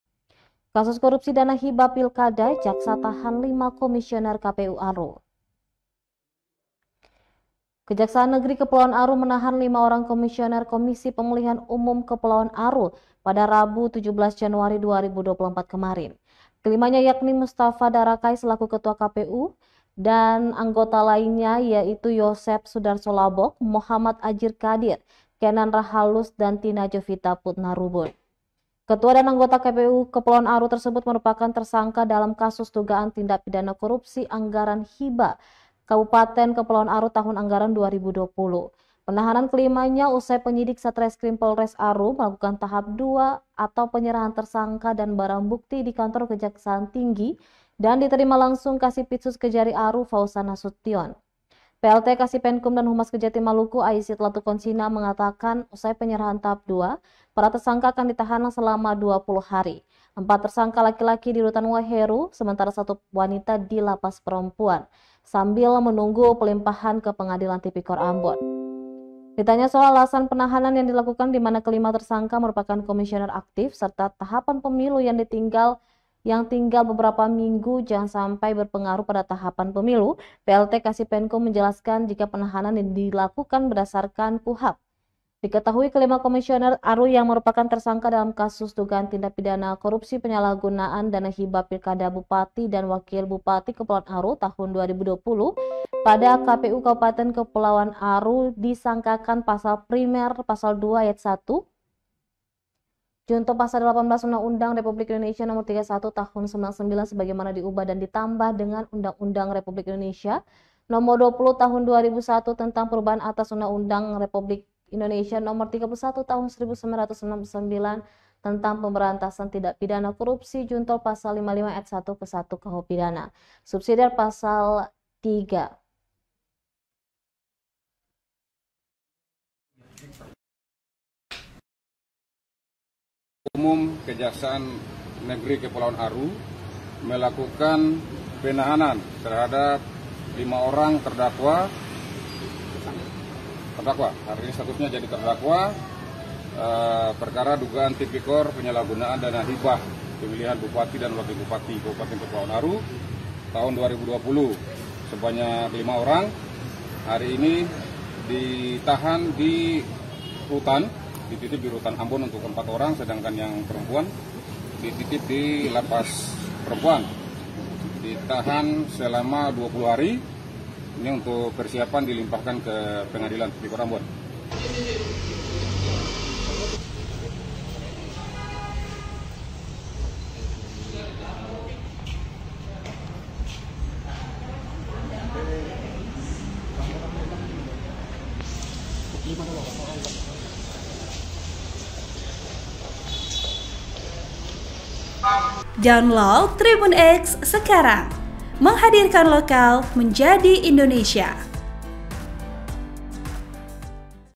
Kasus Korupsi Dana Hibah Pilkada, Jaksa Tahan 5 Komisioner KPU Aru
OA,AMBON, TRIBUNAMBON.COM – Kejaksaan Negeri Kepulauan Aru menahan 5 orang Komisioner Komisi Pemilihan Umum Kepulauan Aru, Rabu 17/1/2024.
Kelimanya yakni Mustafa Darakay selaku Ketua KPU Kepulauan Aru, dan Anggotanya yakni Yoseph Sudarso Labok, Mohamad Adjir Kadir, Kenan Rahalus dan Tina Jovita Putnarubun.
Ketua dan anggota KPU Aru tersebut merupakan tersangka kasus dugaan tindak pidana korupsi anggaran hibah Kabupaten Kepulauan Aru tahun anggaran 2020.
Penahanan kelimanya usai Penyidik Satreskrim Polres Aru melakukan tahap dua atau penyerahan tersangka dan barang bukti di kantor Kejaksaan Tinggi Maluku, Kota Ambon, dan diterima langsung Kasi Pidsus Kejari Aru, Fauzan Nasution.
Plt Kasi Penkum dan Humas Kejati Maluku, Aizit P. Latuconsina mengatakan usai penyerahan tahap II, para tersangka akan ditahan selama 20 hari.
Empat tersangka laki-laki di rutan Waiheru.
Sementara satu wanita di lapas Perempuan sambil menunggu pelimpahan ke PN Tipikor Ambon.
Ditanya soal alasan penahanan yang dilakukan dimana kelima tersangka merupakan komisioner aktif serta tahapan pemilu yang tinggal beberapa minggu jangan sampai berpengaruh pada tahapan pemilu, Plt Kasi Penkum menjelaskan jika penahanan yang dilakukan berdasarkan Kuhap.
Diketahui, Kelima komisioner Aru yang merupakan tersangka dalam kasus dugaan Tindak Pidana Korupsi Penyalahgunaan Dana Hibah Pilkada Bupati dan Wakil Bupati Kepulauan Aru Tahun 2020 pada Komisi Pemilihan Umum Kabupaten Kepulauan Aru.